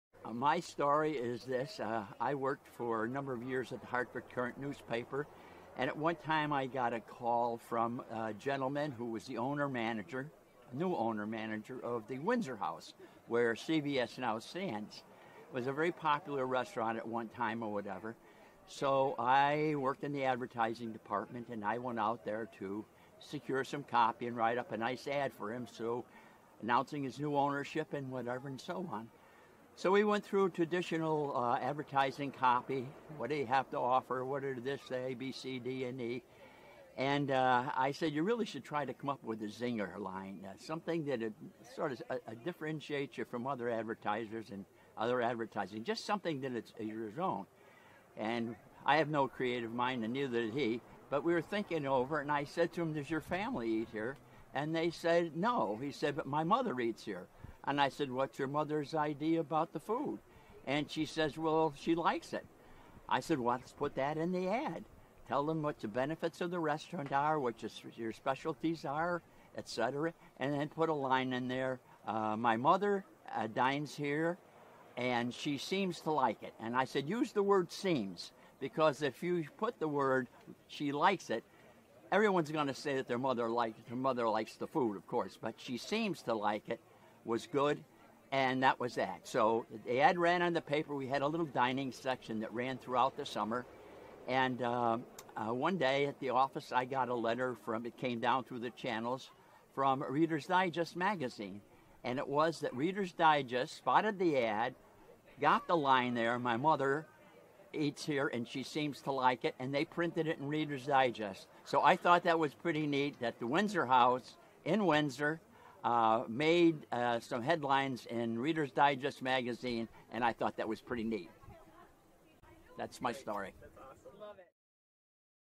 Bill Sullivan - Share Your Windsor Story (Clip)
Recorded on August 15th, 2021, at the Brown's Harvest Festival
Interviewee: William Sullivan
Interviewer: Michelle Tom

Special thanks to WIN-TV for filming all of our 'Windsor Stories'.